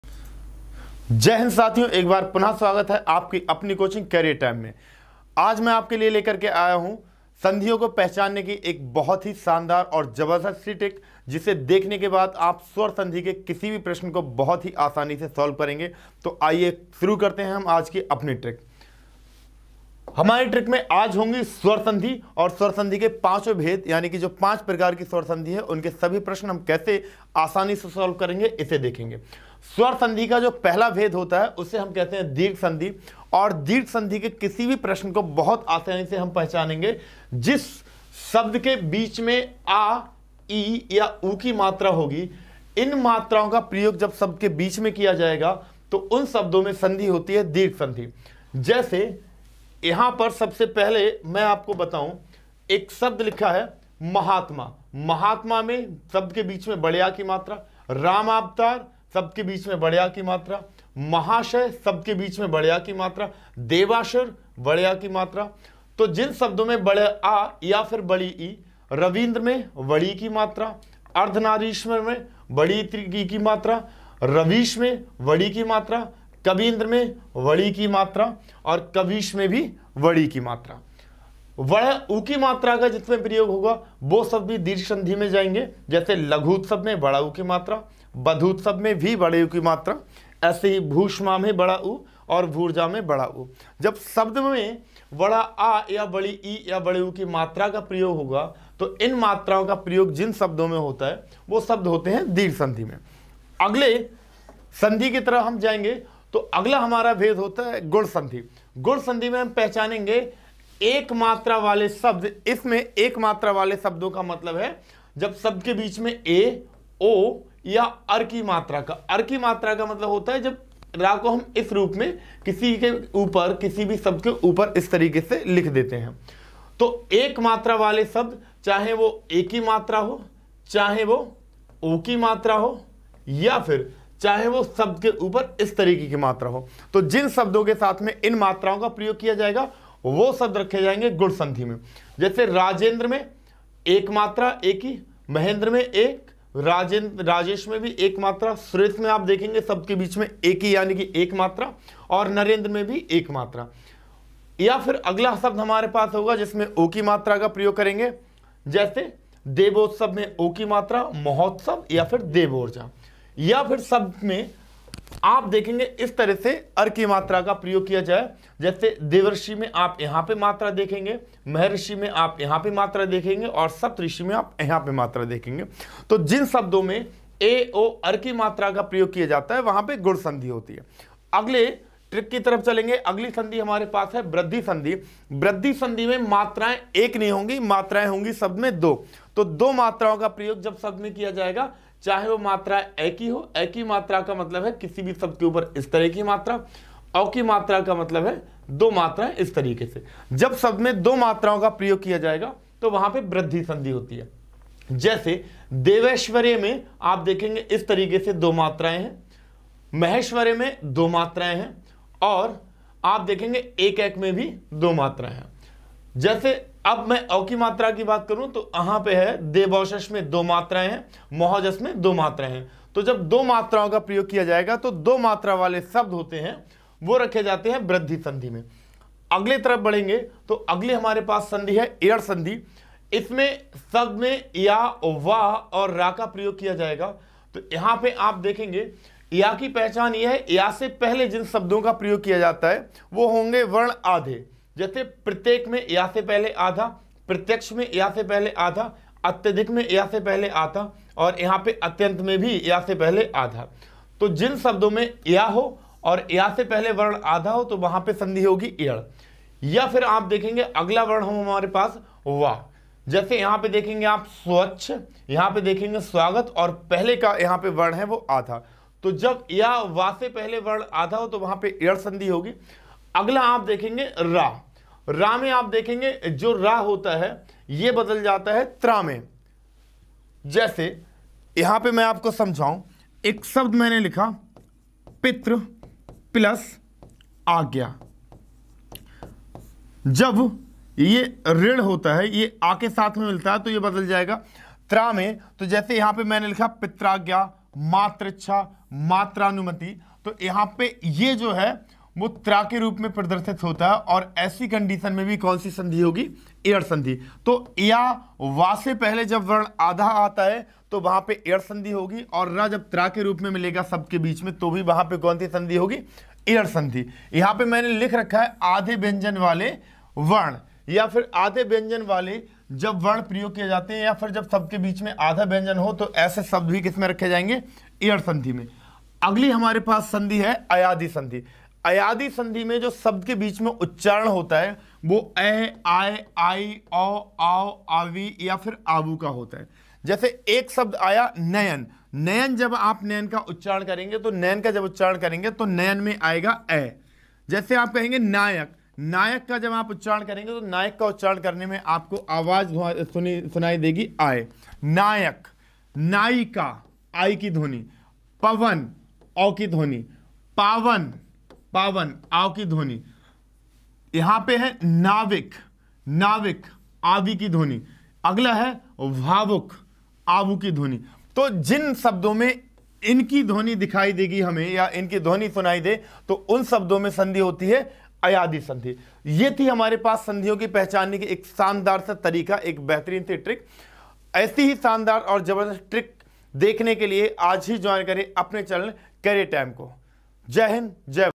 Tricks Special For All Govt. Exam | Hindi Tricks | Sandhi (संधि) Hindi By Dharampal Sir
Career Time Online Coaching के साथ और भी बेहतर तैयारी के लोए अभी फॉर्म भरें

जुडे रहिए Career Time Online Coaching के साथ और सीखते रहिए